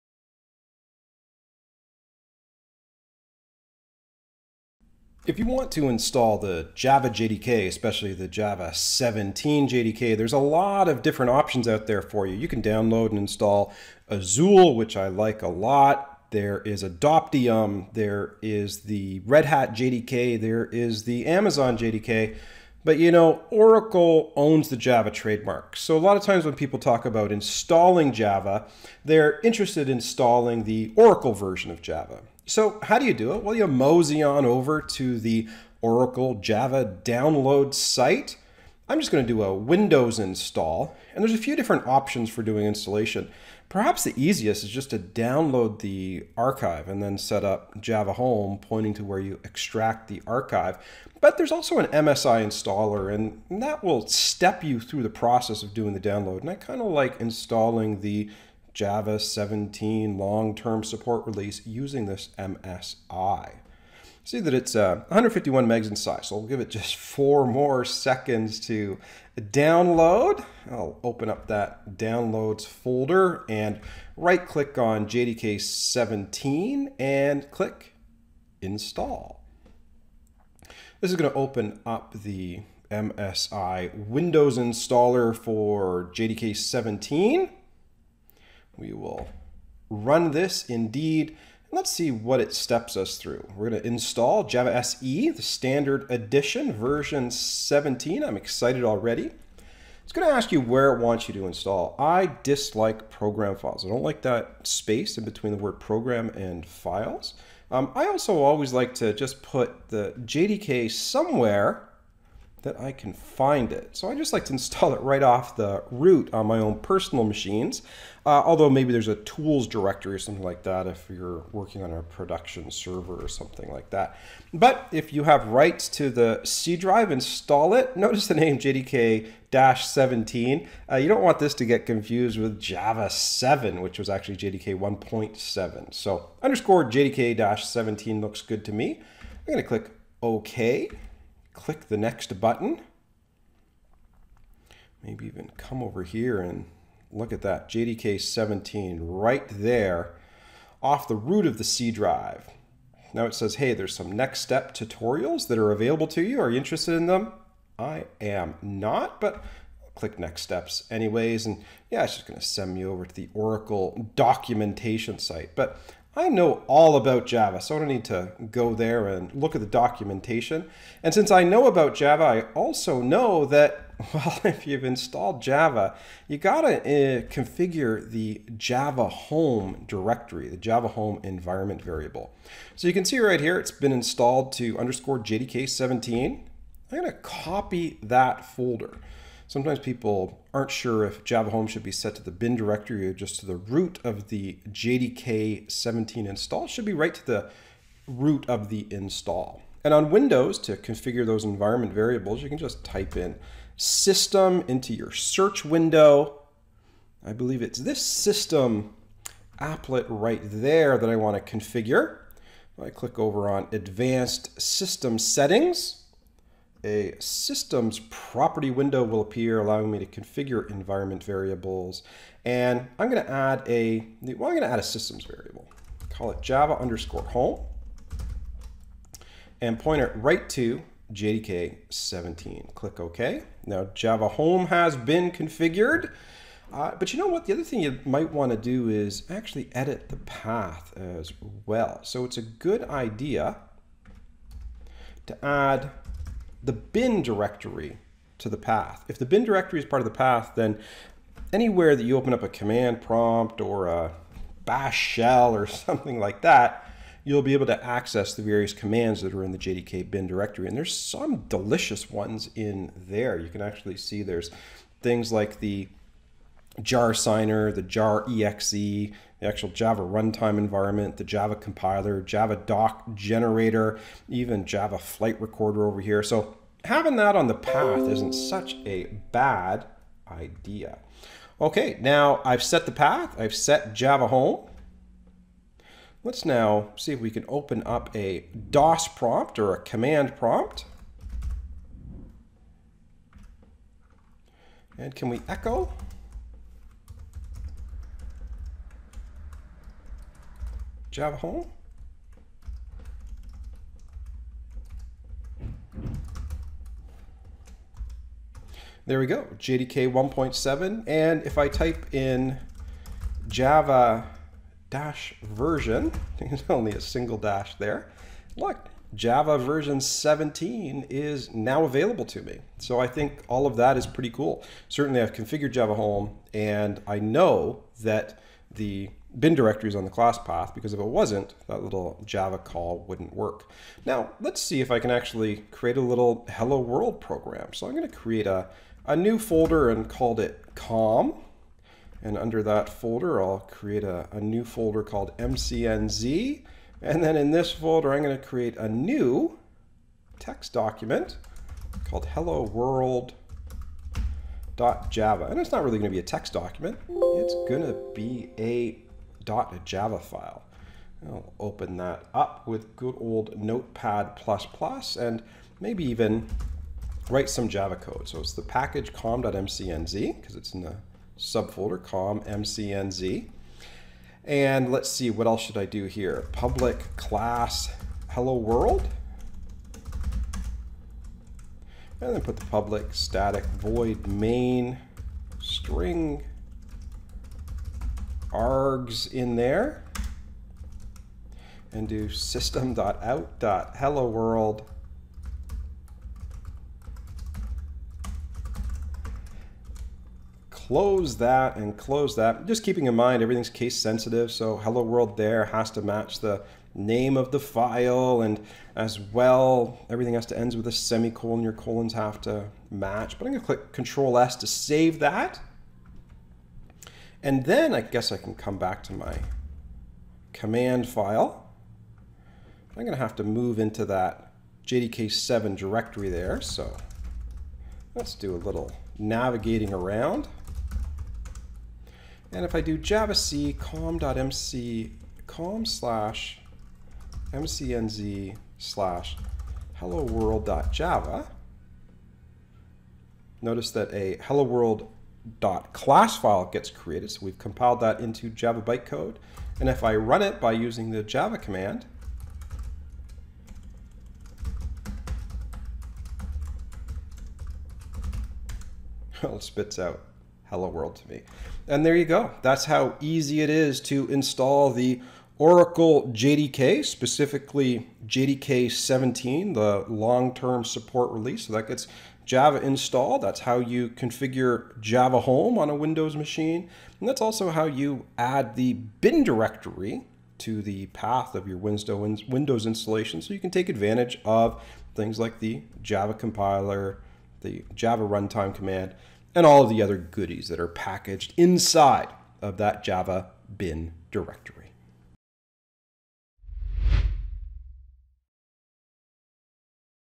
How to Install Oracle JDK and Set JAVA_HOME
Follow these steps to install the Oracle JDK for Java 17 at set JAVA_HOME to begin your development.